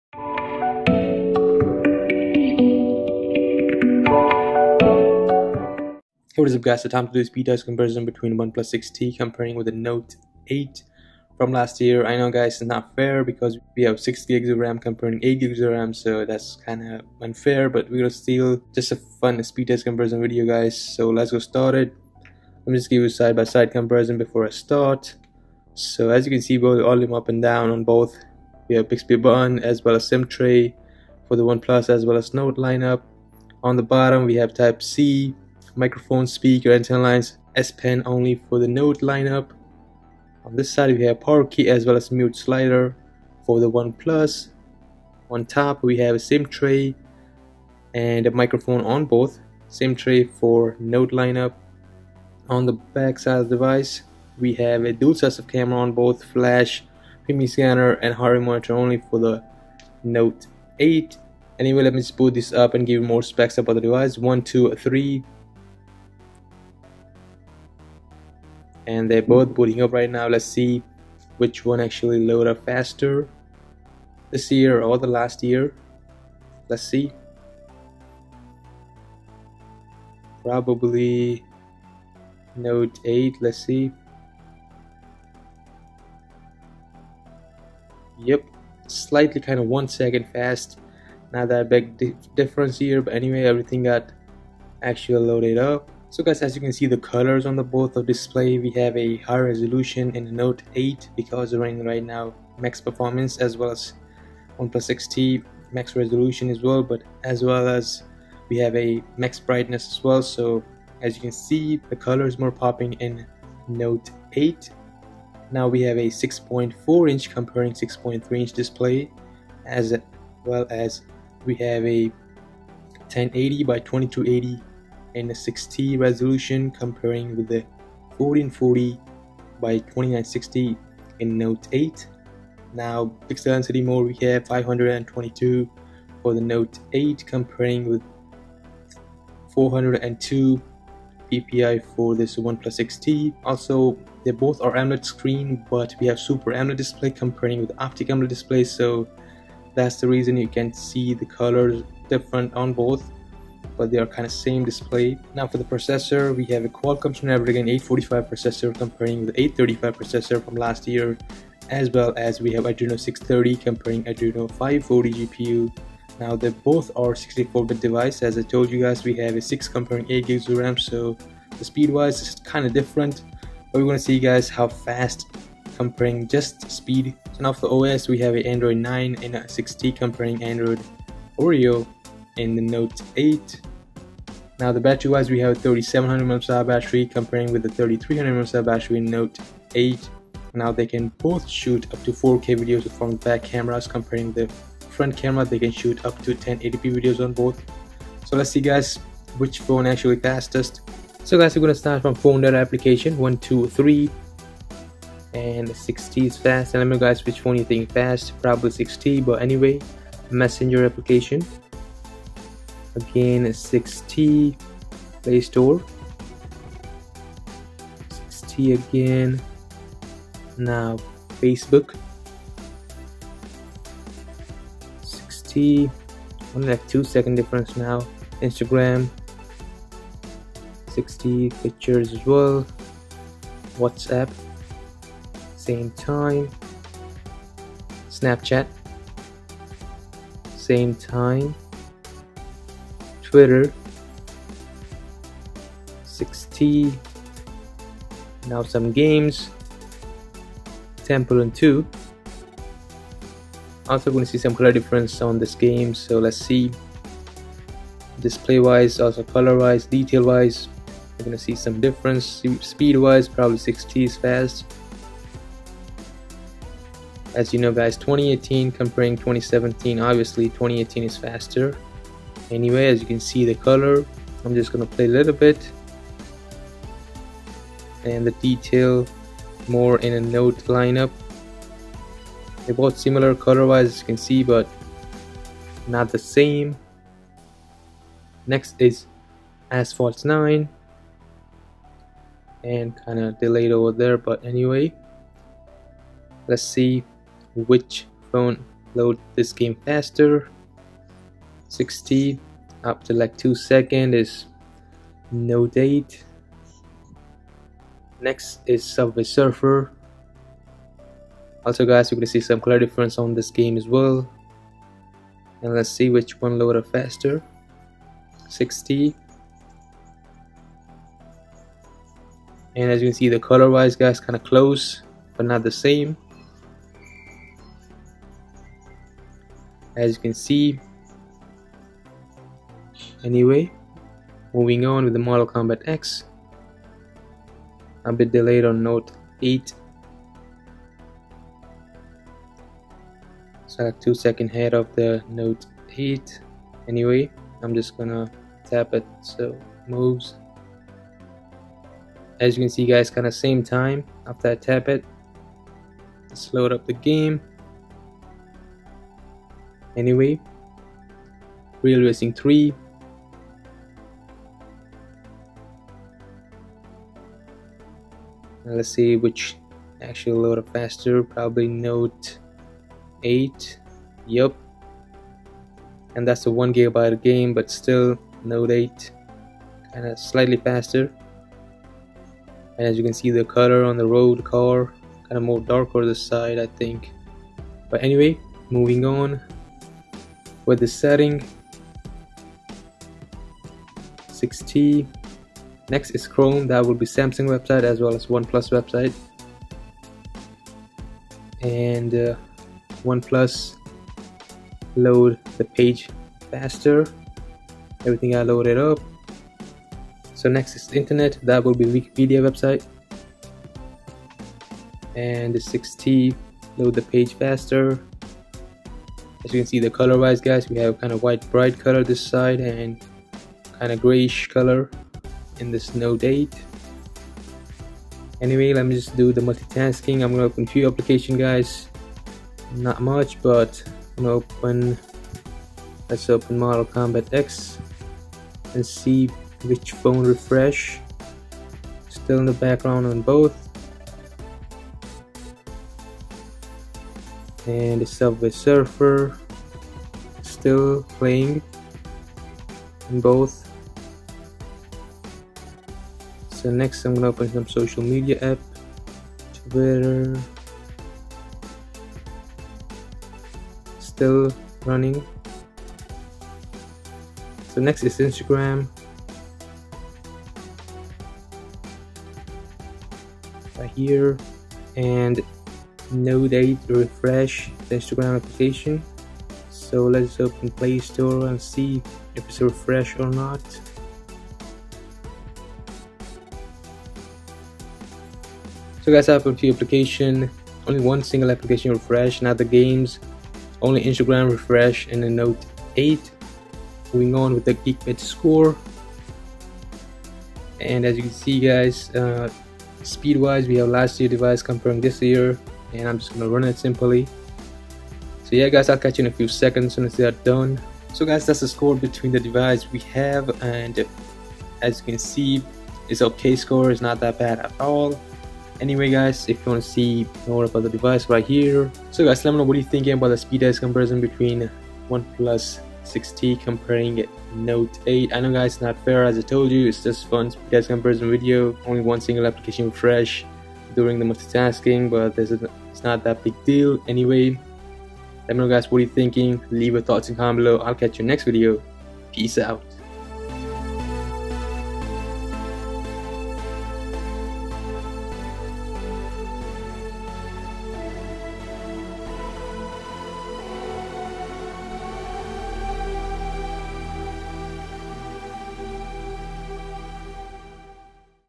OnePlus 6T Vs Samsung Galaxy NOTE 8 Speed Test !!!
OnePlus 6T vs Samsung Galaxy NOTE 8 Speed Test. Can the NOTE 8 Will slay the beast? Let's find out.

OnePlus 6T Vs Samsung Galaxy NOTE 8 Speed Test. A full speed comparison between Snapdragon 845 Chipset (8GB RAM) Vs Snapdragon 835 Chipset (6GB RAM) including apps opening, RAM Management, browsing, boot up, & geek bench 4 score (2018 vs 2017). Which is faster?

▶︎(Give your friends $5 to spend at Amazon.)(▶︎▶︎SUMIT3611N◀︎◀︎)